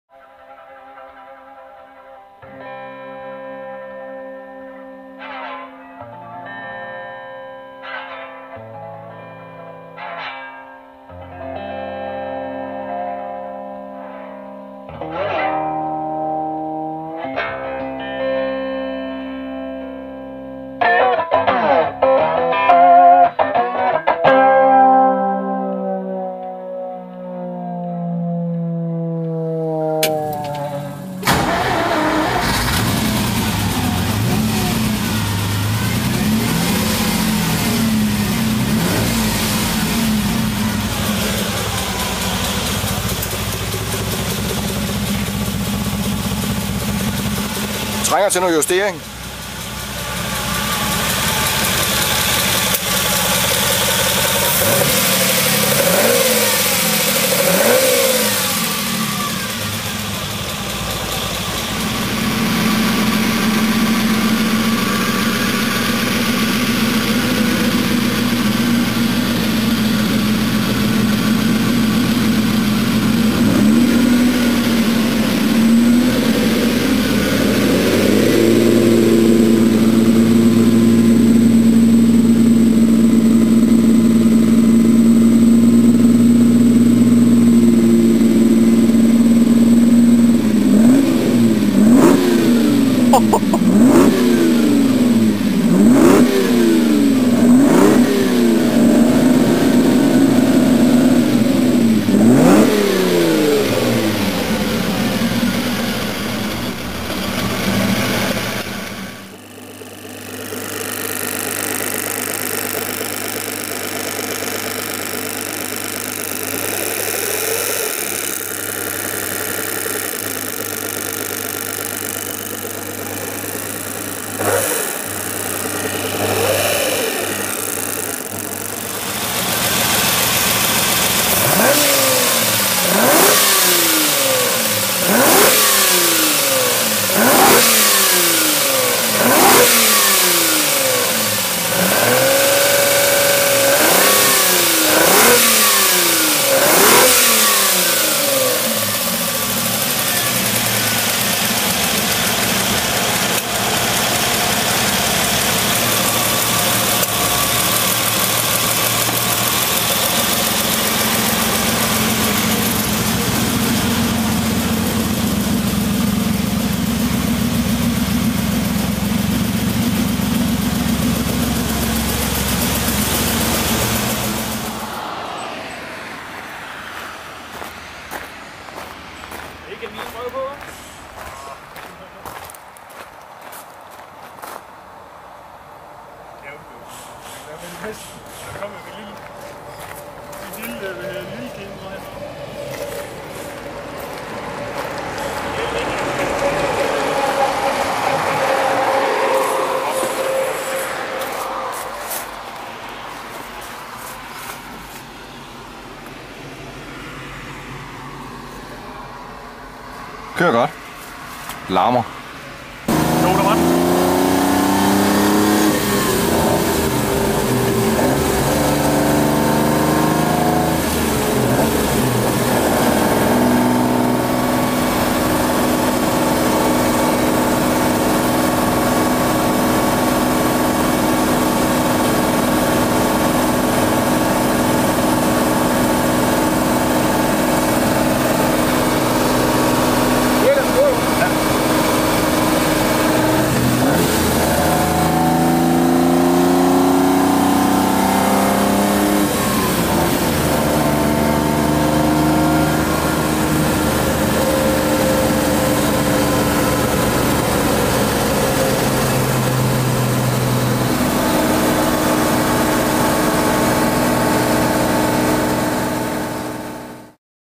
Ferrari 250 GTO
Steffen Madsen - This beautiful car came in for a tune-up of the marvolous V12 engine